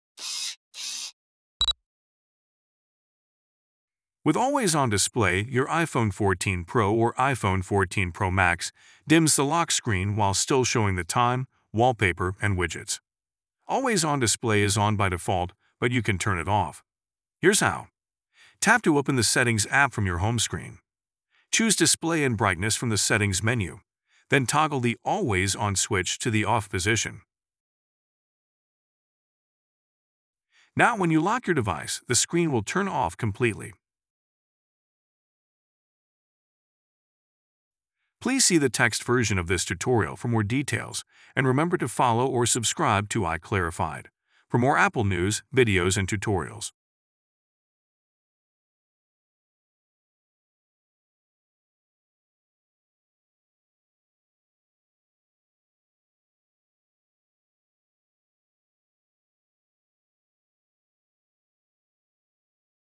How to Turn Off Always On Display on iPhone 14 Pro
iClarified instructions on how to turn off the always on display on your iPhone 14 Pro and iPhone 14 Pro Max.
Save big on Apple products and accessories!